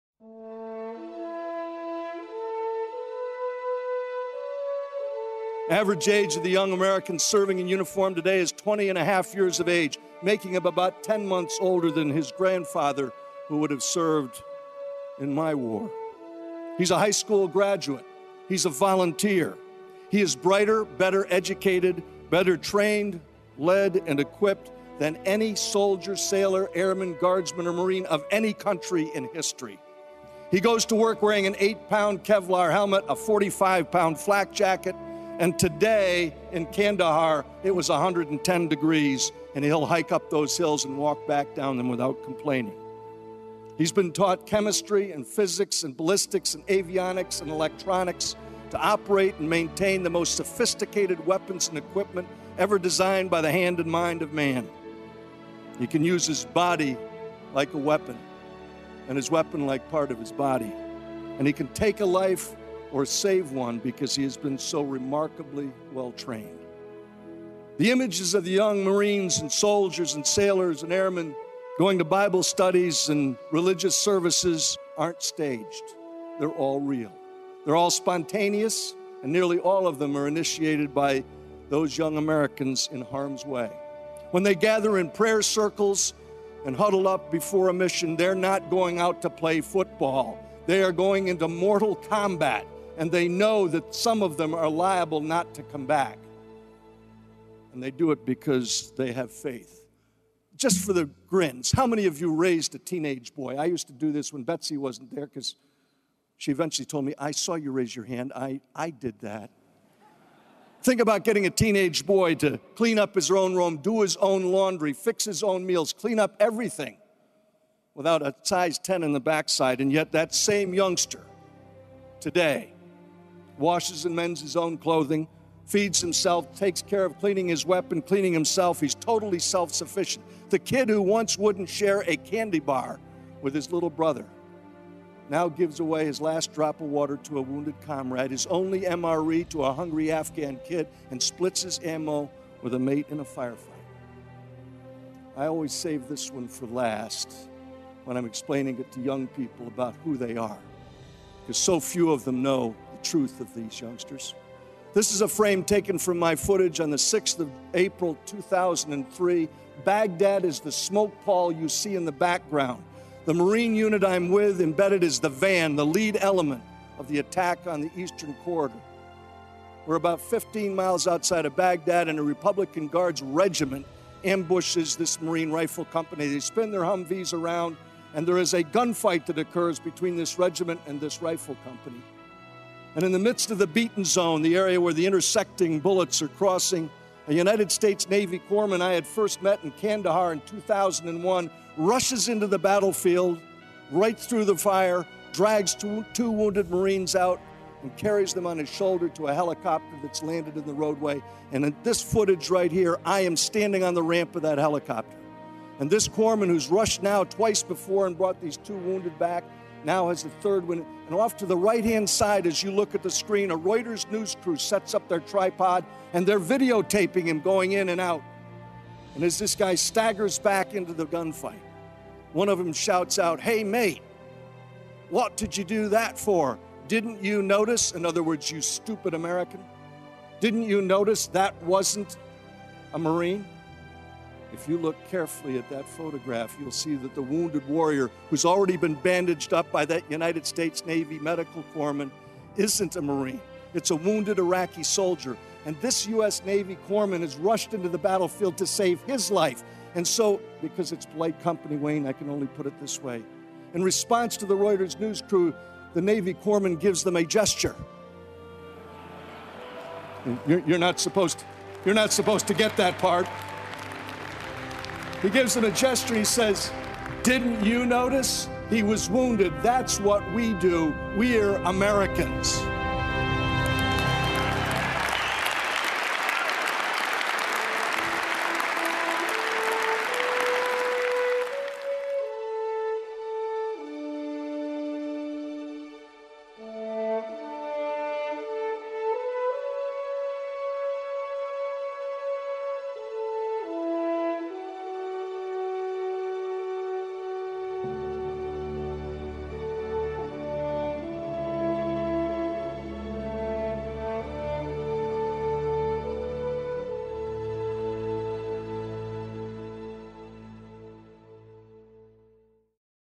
Oliver North, US Troops Veterans Day Video
This video tribute on the front lines is the story of true patriots, told by Lieutenant Colonel Oliver North at the NRA Annual Meetings in 2009. It's a stirring profile of the spirited, courageous guardians of our precious freedoms.  America's best ...